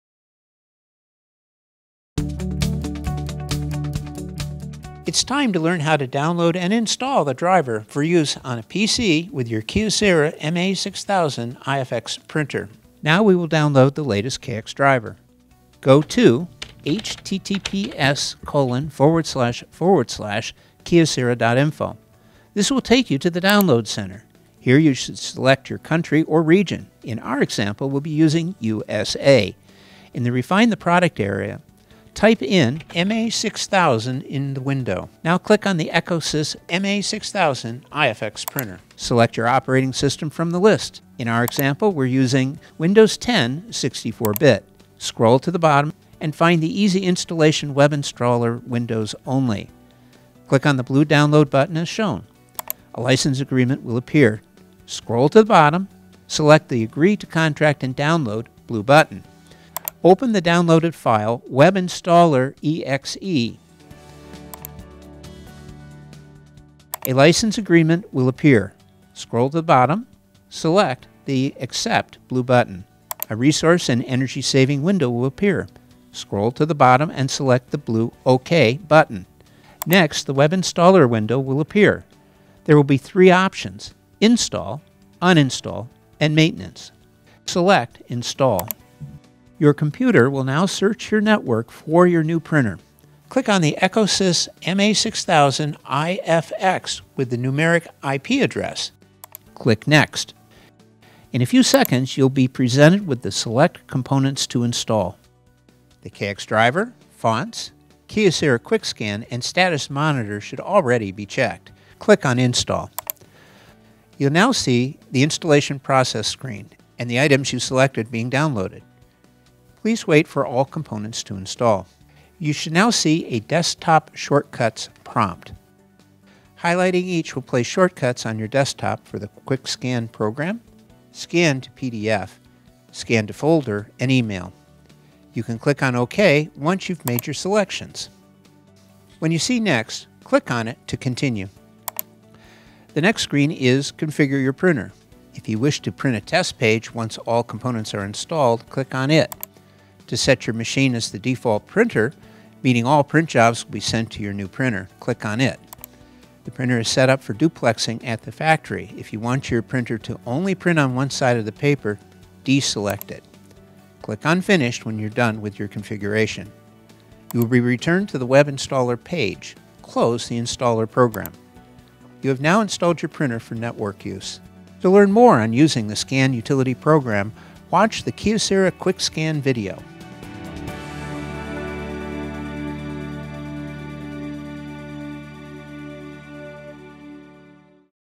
ECOSYS MA6000ifx/ MA5500ifx/ MA4500ix/ MA4500ifx Install Printer Driver for Windows
Follow our step-by-step guide to download and install the KX Printer Driver for Windows and configure your Kyocera ECOSYS MA6000ifx/ MA5500ifx/ MA4500ix/ MA4500ifx printer for network use.